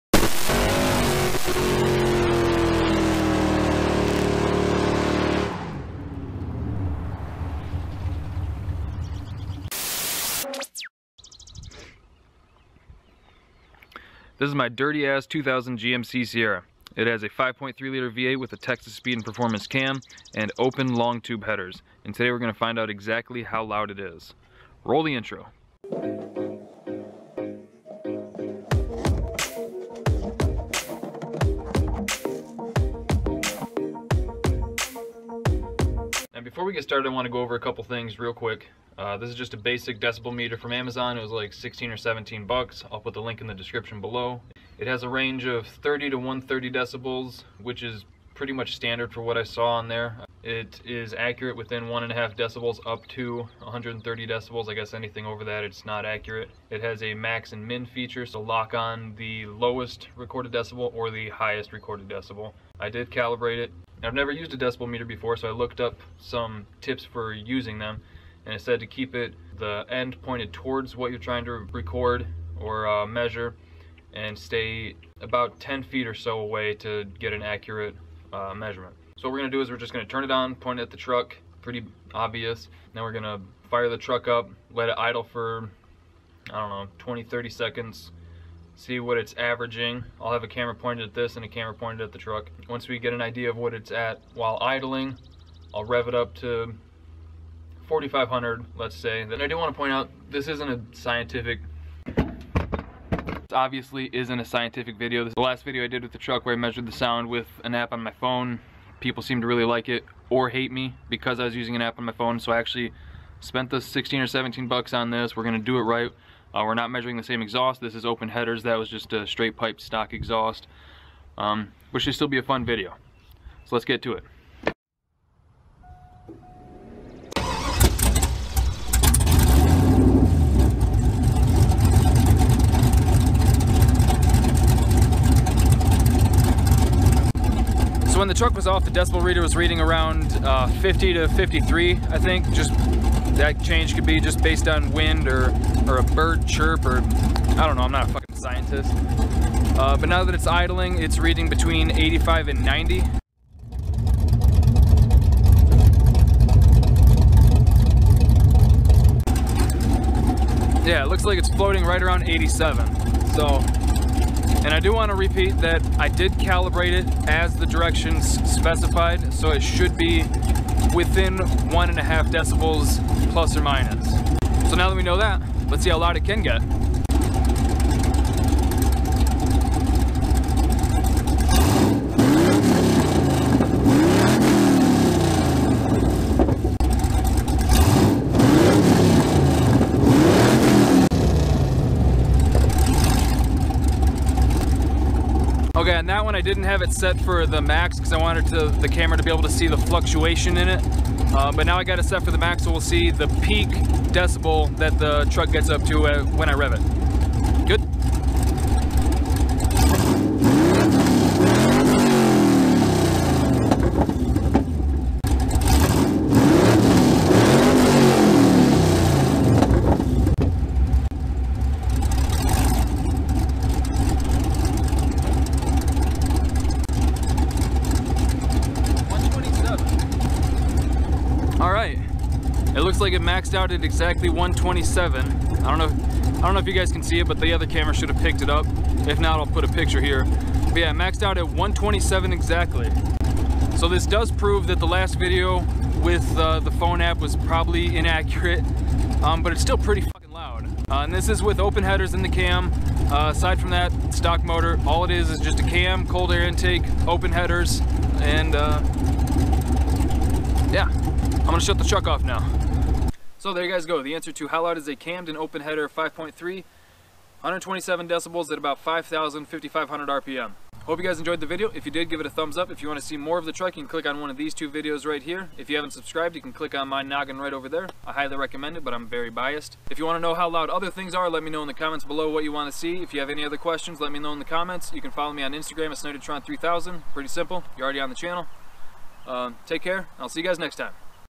GMC Sierra with stage 3 cam & open headers dB test
In this video, I use a proper decibel meter to measure how loud my GMC Sierra is. It has a 5.3 with a Texas Speed 216/220 cam and PaceSetter 1 3/4 long tube headers. The ONLY other piece of exhaust is 3inch straight pipe approximately 18 inches long on each header. When revving the engine, I only took it up to ~5,500RPM.

This video was meant mostly as fun, but it could definitely give someone an idea of what to expect from this combination.

The music I use in my videos is made by these rad people: